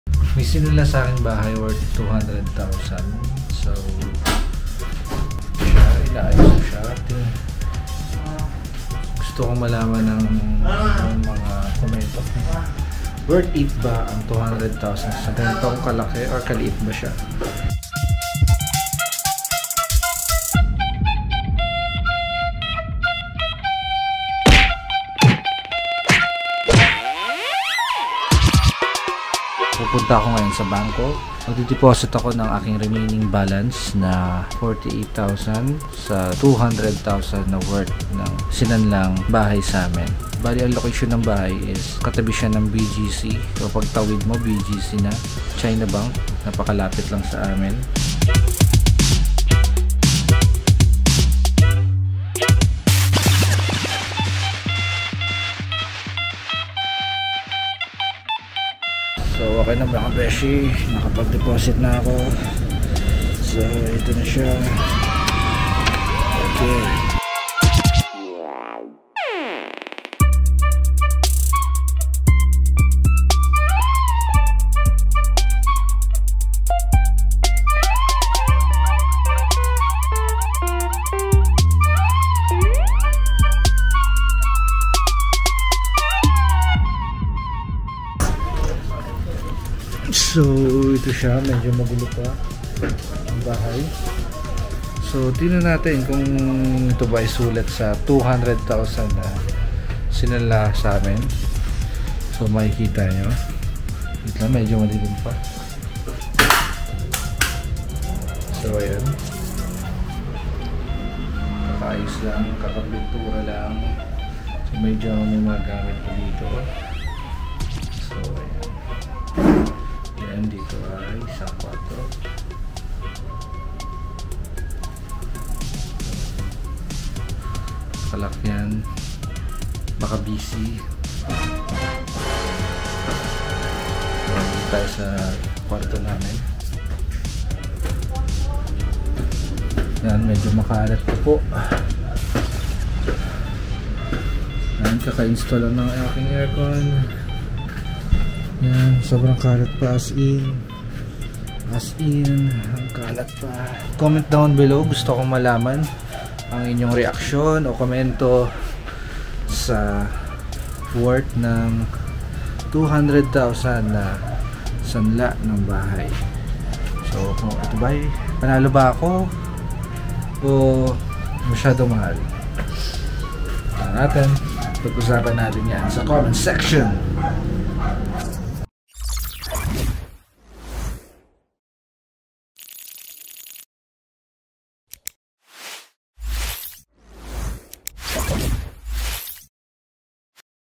P200,000 Sangla-tira beside BGC! | Pisobilities
Panalo ba ang P200,000 ko sa Sangla-tira beside BGC? malaki din ba ang bahay for P200,00? Pag-usapan natin yan sa Comment Section!